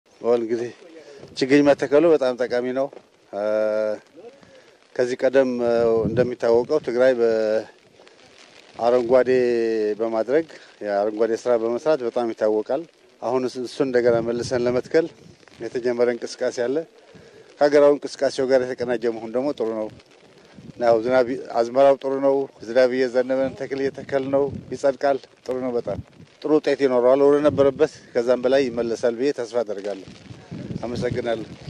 የችግኝ ተከላው ከአገራዊ እንቅስቃሴው ጋር መቀናጀቱ ጥሩ ነው - ሌተናል ጀነራል ፃድቃን ገብረትንሳኤ
የችግኝ ተከላው ከአገራዊ እንቅስቃሴው ጋር መቀናጀቱ ጥሩ ነው - የትግራይ ክልል ጊዜያዊ አስተዳደር ምክትል ርዕሰ መሥተዳድር ሌተናል ጀነራል ፃድቃን ገብረትንሳኤ